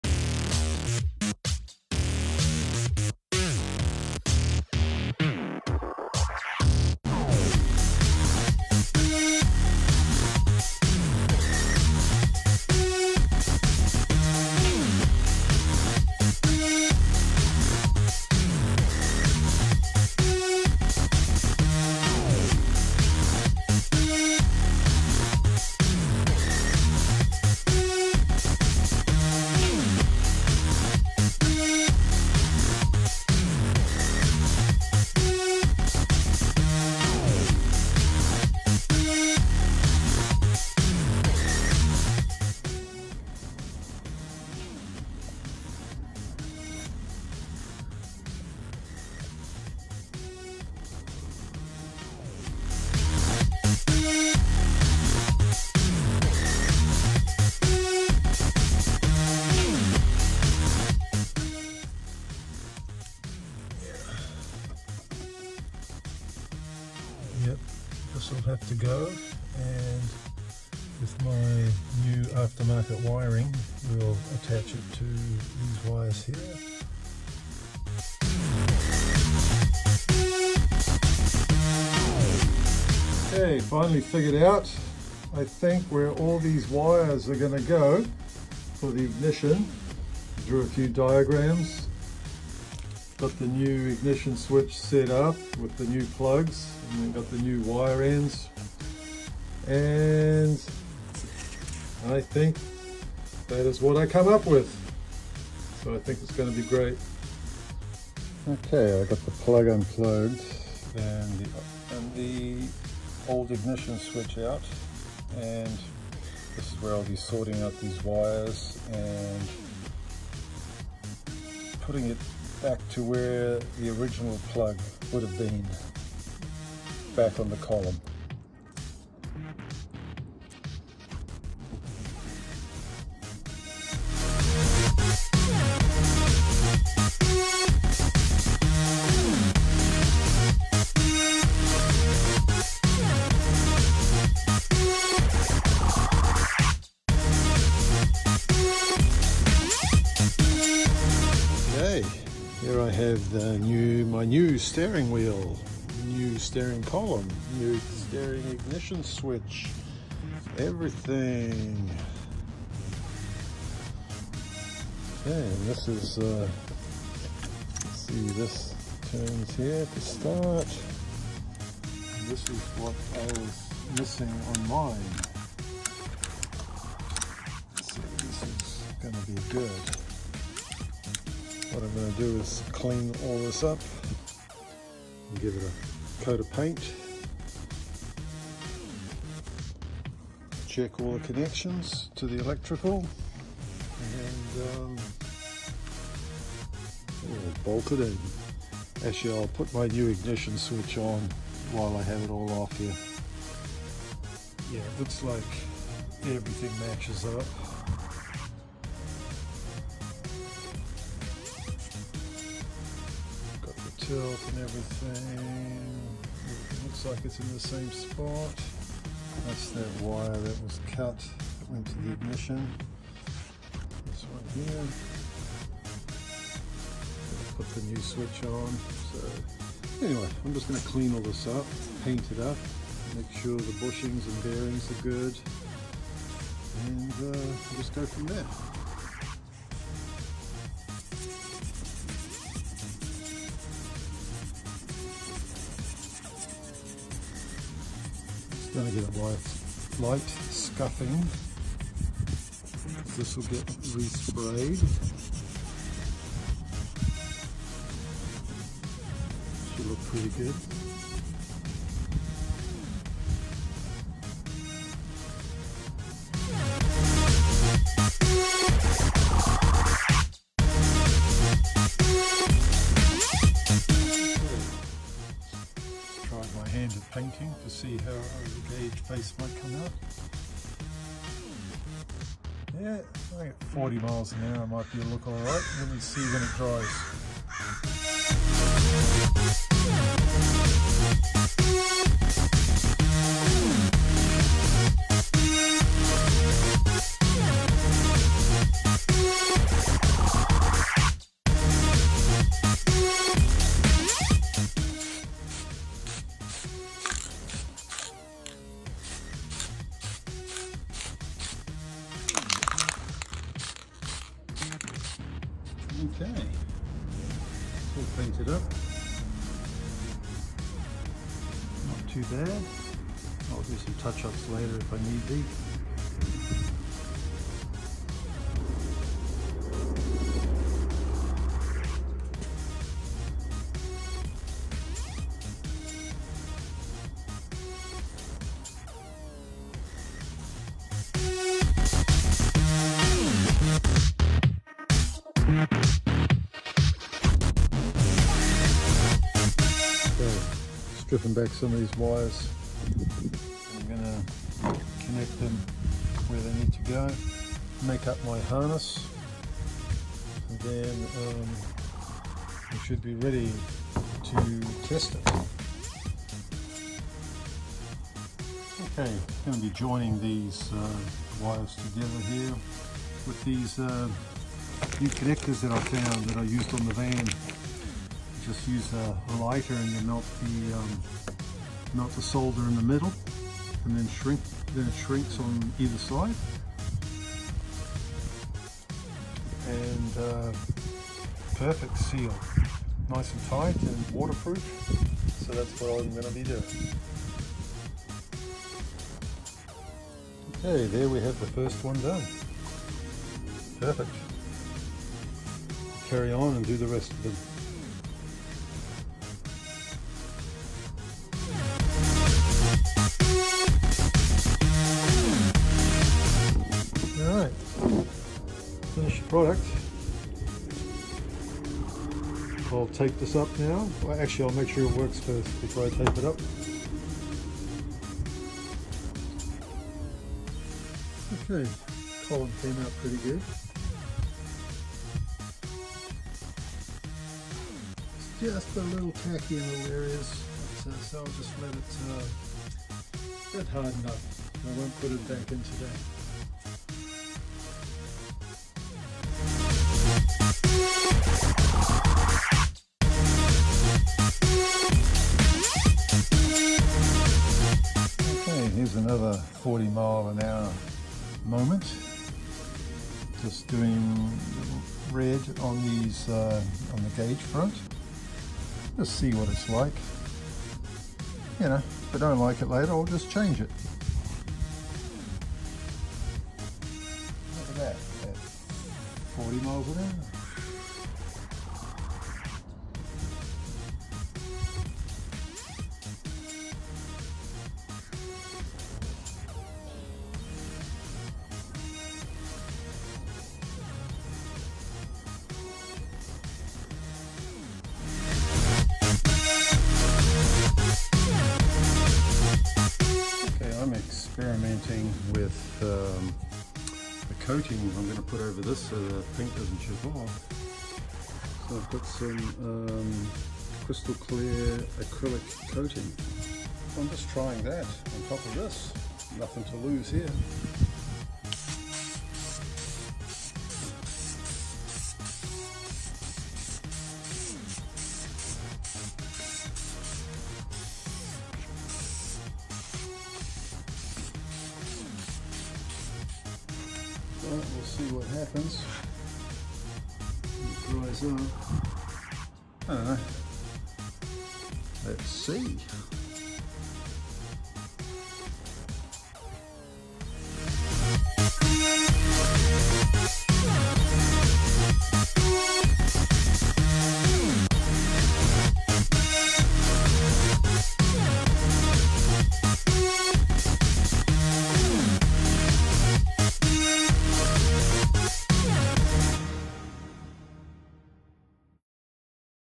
1979 Dodge D100. Stepside. Part 11.
On this video, I moved right along repairing the wiring harness to the ignition switch also cleaning up and painting the steering column and painting the new Dash gauge face facia, and experimenting also with customizing it.